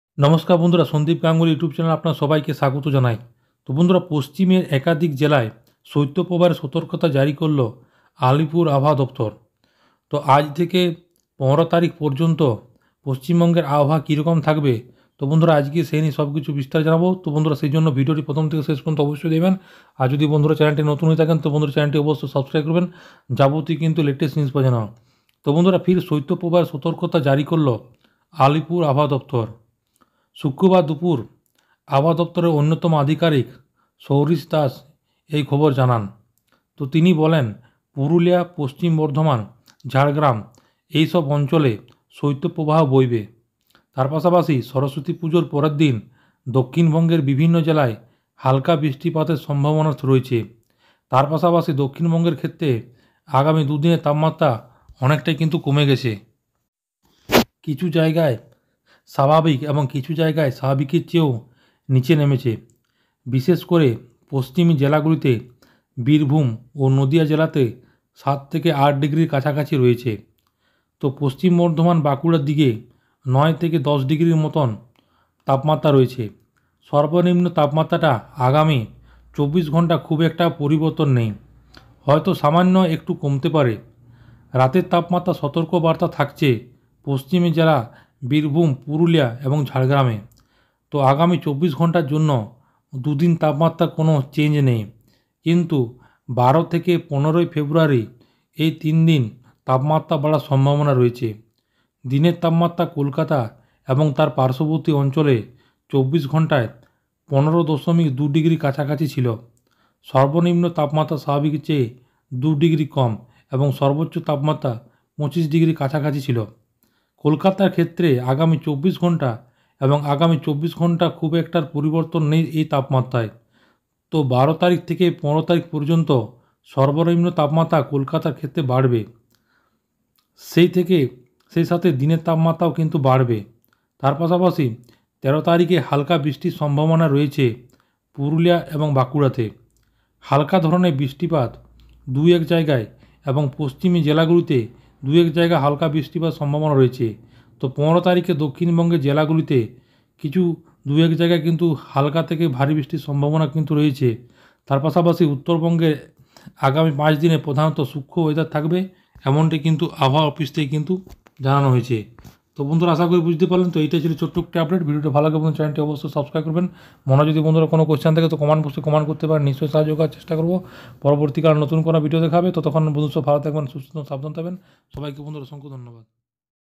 West Bengal Weather Today: আরও নামলো তাপমাত্রা, শৈত্য প্রবাহের পরিস্থিতি পশ্চিমের জেলায়
Bengal Weather Today: আরও নামলো তাপমাত্রা, শৈত্য প্রবাহের পরিস্থিতি পশ্চিমের জেলায়
Weather update in westbengal: ফের গরমে পুড়বে বাংলা। আগামী ৪৮ ঘণ্টায় কলকাতায় তাপমাত্রার পারদ ৪০ ডিগ্রি ছাড়াতে পারে বলে পূর্বাভাস দিয়েছে আবহাওয়া (Weather) দফতর। উত্তরবঙ্গের তিন জেলাতেও চলবে তাপপ্রবাহ। আর এ-সবের নেপথ্যে রয়েছে আসন্ন ঘূর্ণিঝড় মোকা! এমনটাই জানাচ্ছে আবহাওয়া দফতর।